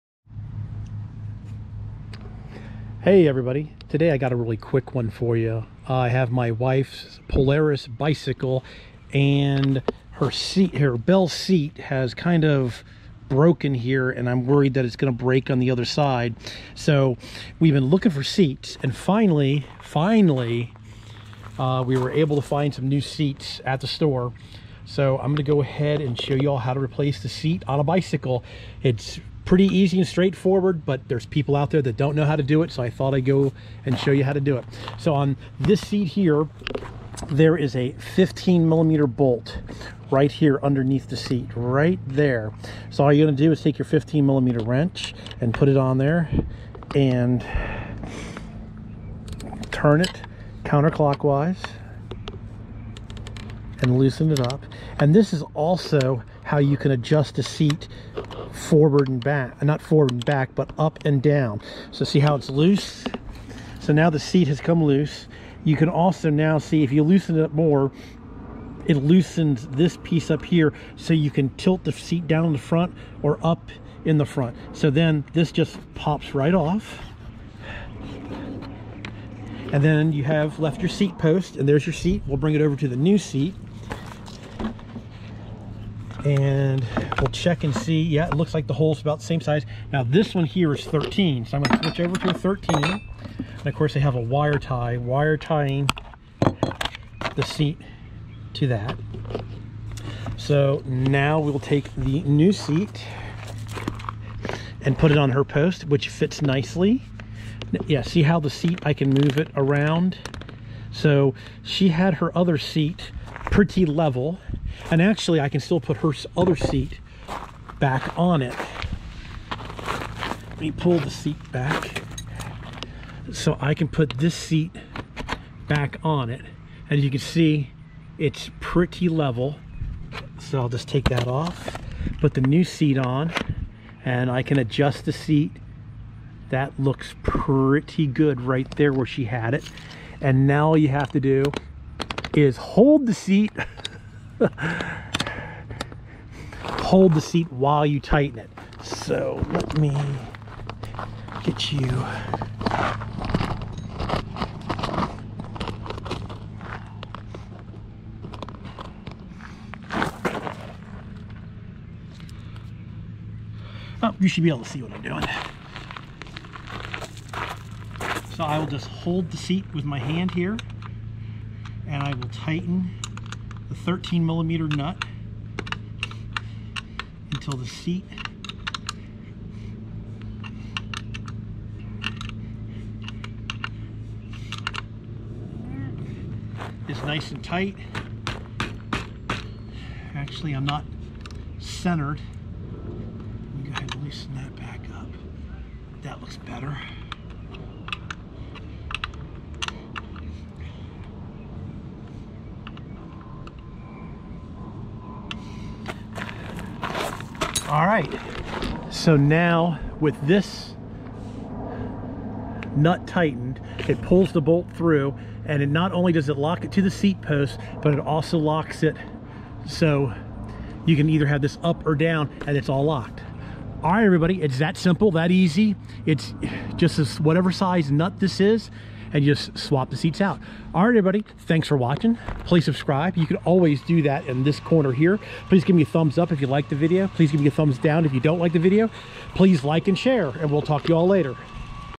How To Change A Bicycle Seat
Just a fast one on how the change your seat on a bicycle.

This video was made as a demonstration on how to repair things and should not be done with out the proper training and safety equipment. Please if you don't have the ability to do these demonstrations, don't try and get a professional to do it for you.